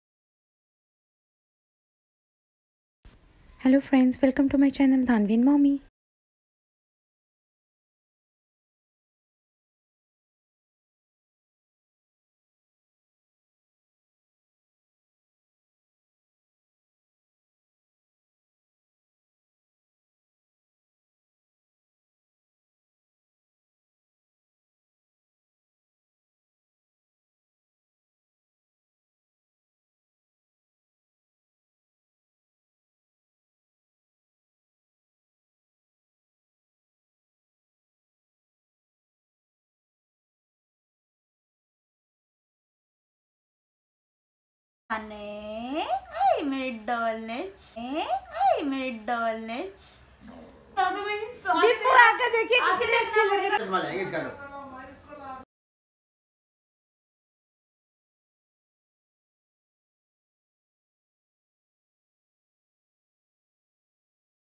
How to create photoshoot with home stuff || Dhaanvi favourite 🍟 items || @Nataashasinghal 🥰🥳
How to create photoshoot with home stuff || Dhaanvi favourite items || Dhaanvi smiles very first time during her photoshoot ||